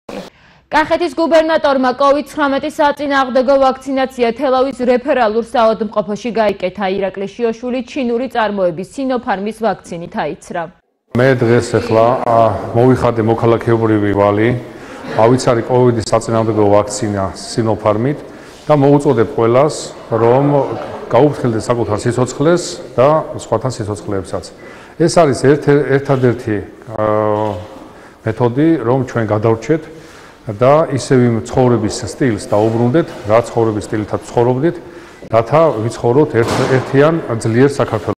კახეთის გუბერნატორი ჩინური ვაქცინით აიცრა
კახეთის  გუბერნატორმა  კოვიდ 19-ის  საწინააღმდეგო ვაქცინაცია თელავის რეფერალურ საავადმყოფოში გაიკეთა. ირაკლი შიოშვილი   ჩინური წარმოების სინოფარმის ვაქცინით აიცრა.